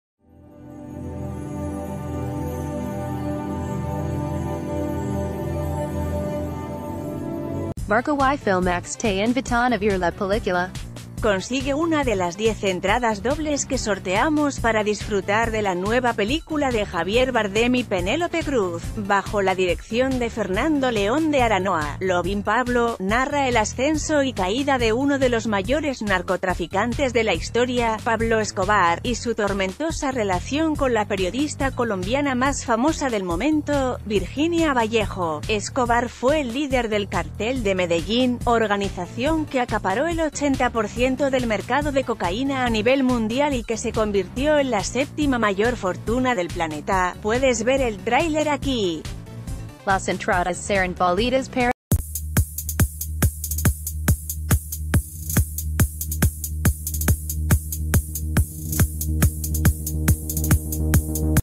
Noticias de última hora | Mas Marca
MARCA y Filmax te invitan a ver la película LovingPablo. Consigue una de las 10 entradas dobles que sorteamos para disfrutar de la nueva película de Javier Bardem y Penélope Cruz, bajo la dirección de Fernando León de Aranoa. 'Loving Pablo' narra el ascenso y caída de uno de los mayores narcotraficantes de la historia, Pablo Escobar, y su tormentosa relación con la periodista colombiana más famosa del momento, Virginia Vallejo. Escobar fue el líder del cartel de Medellín, organización que acapa...